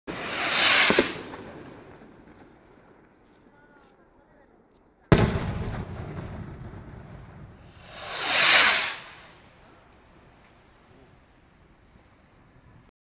Salvar a Praia de Covas lanza fuegos para celebrar el trasvase de arena
El colectivo Salvar a Praia de Covas lanzó este sábado fuegos para celebrar el trasvase de arena en la playa.